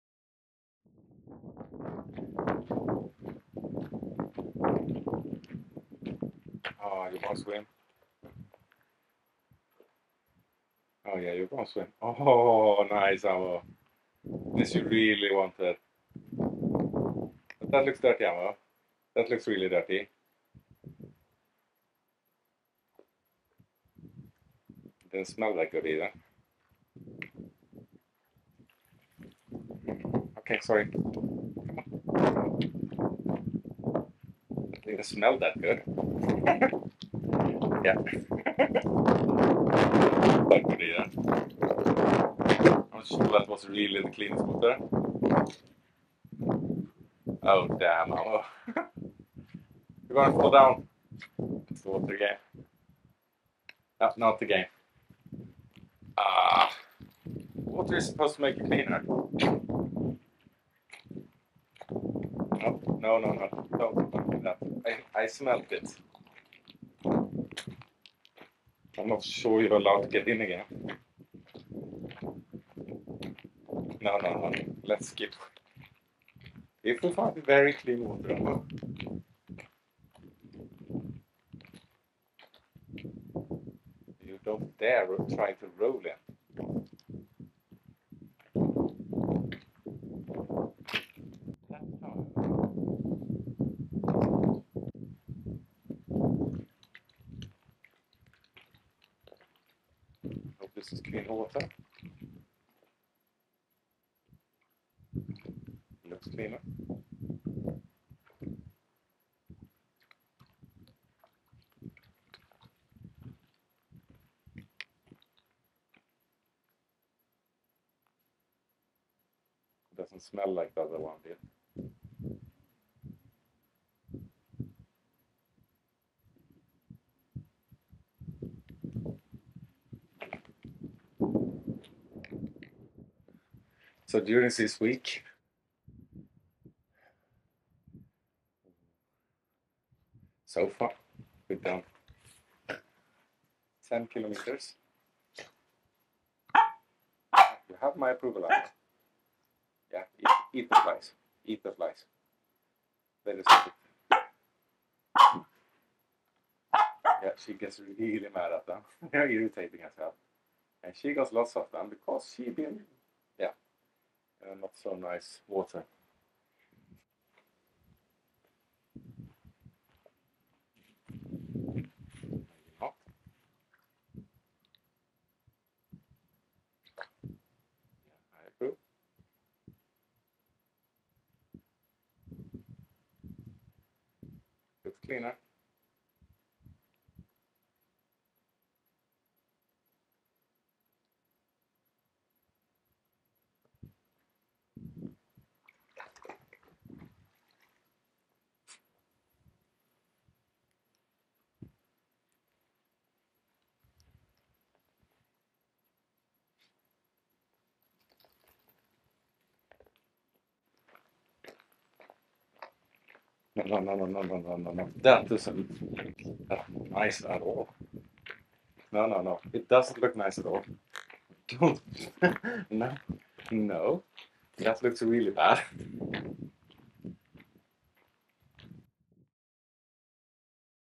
Dirty Water - Dirty Dog
Aamu finds some really foul smelling water and naturally jumps in. Daddy is obviously not as happy about it and tries to get her into cleaner water.

20210730B-1 Aamu Briefs